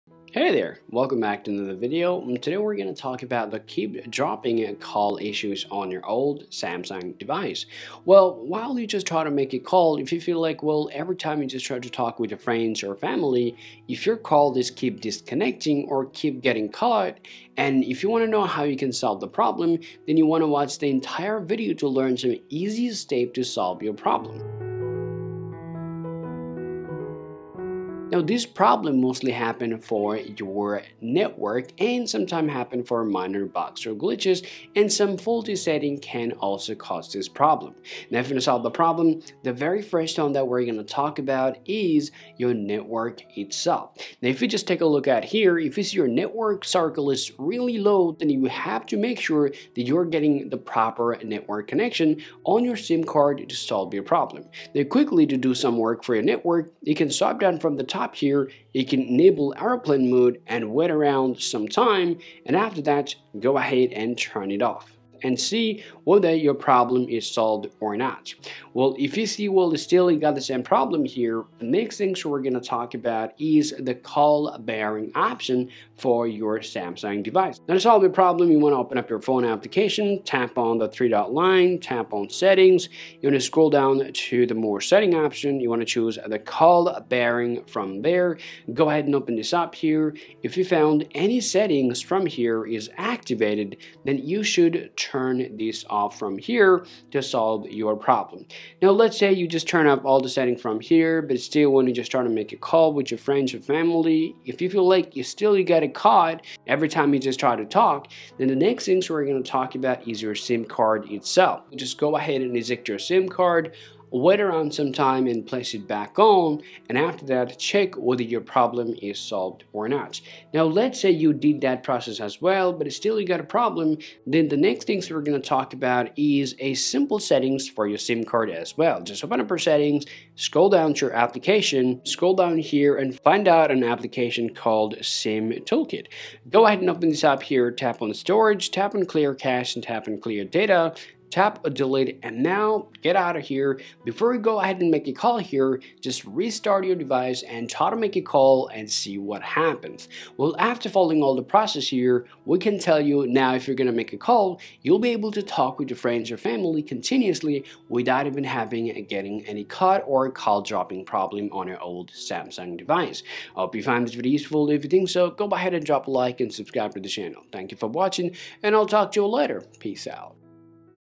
How To Fix Calls Disconnecting Automatically On Samsung! [Random Dropping]
When you are on a call on an old Samsung Phone suddenly gets disconnected randomly and it is happening more and more. Here we will show you the 4 best solutions to fix call disconnecting at random problems on old Samsung Phones easily.

0:00 What this video is about
0:28 Why this happens
0:38 Solution 1: Check and restart Network
1:09 Solution 2: Turn Off Call Barring
1:48 Solution 3: Eject and Reinsert SIM Card
2:04 Solution 4: Clear Cache and Data